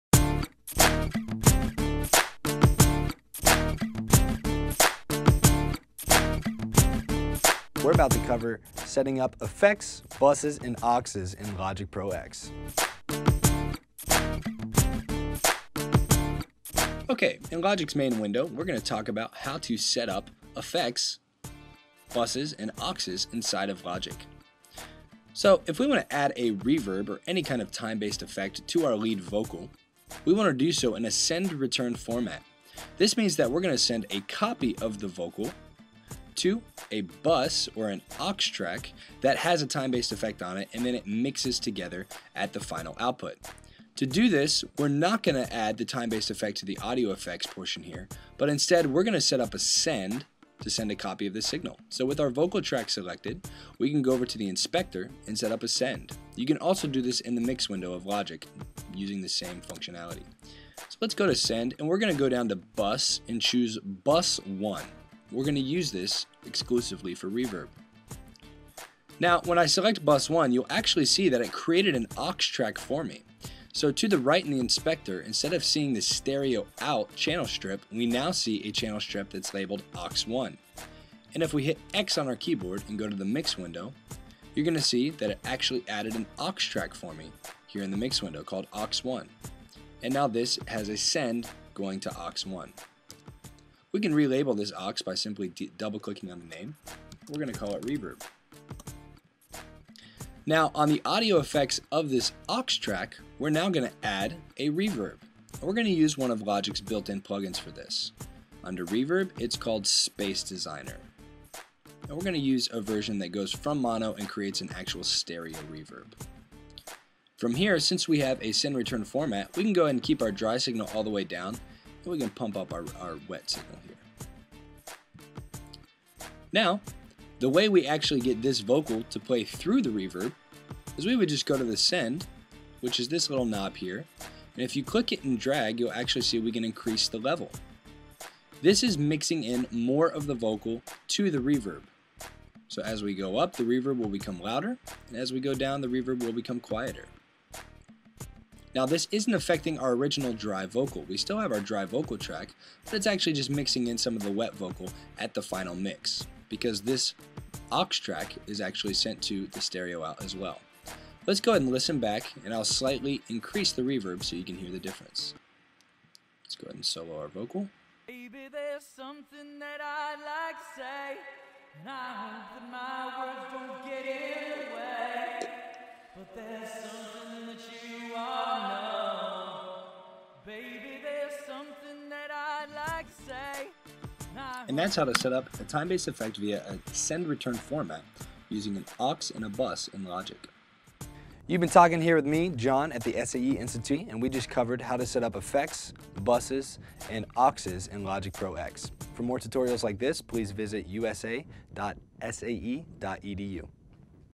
EFFECTS, AUXES, AND BUSES IN LOGIC PRO X V2 1
SAE Institute Audio Instructor Jon Wygant is back this week to show you how to set up effects, auxes and buses in Logic Pro X.